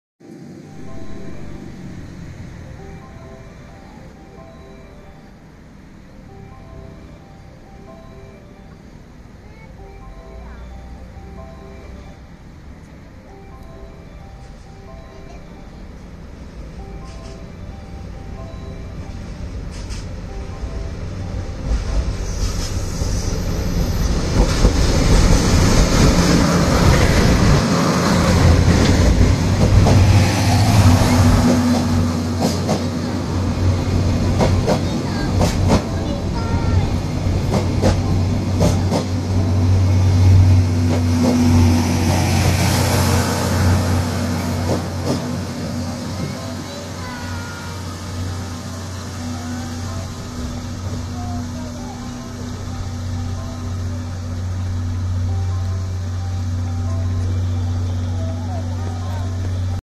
【40周年記念特製マーク】 団体臨時列車 サロンカーなにわ 金沢行 おごと温泉駅 到着
2023年11月3日
１番ホーム
9:44着